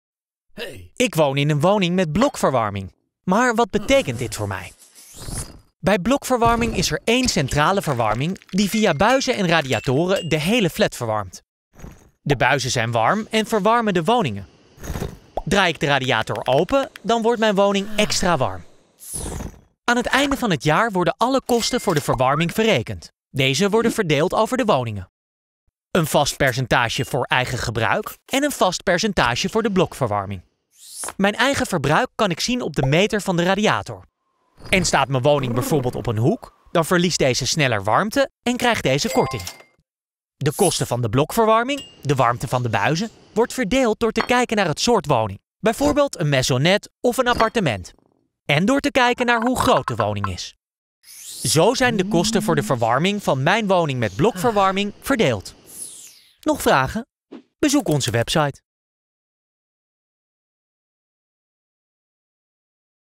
Hoe werkt blokverwarming
Hoe werkt blokverwarming? In deze animatie leggen we kort uit hoe.